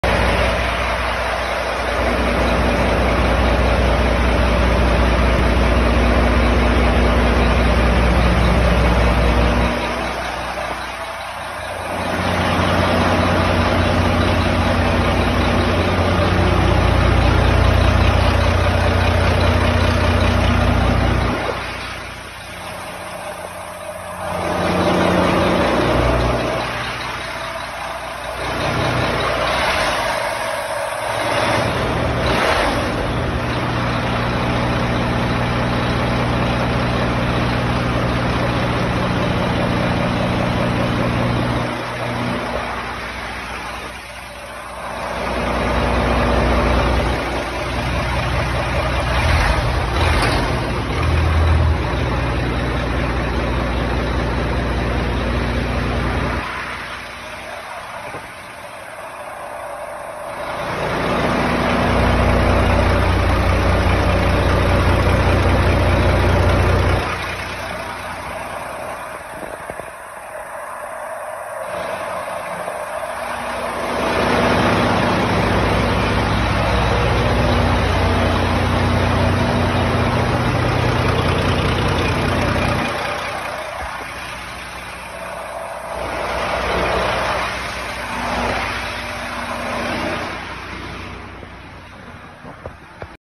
Pair of John Deere 6125 Diesel Marine Propulsion Engines.
Very nice John Deere 6125 diesel engines used for marine propulsion. Marine spec’d, very strong runners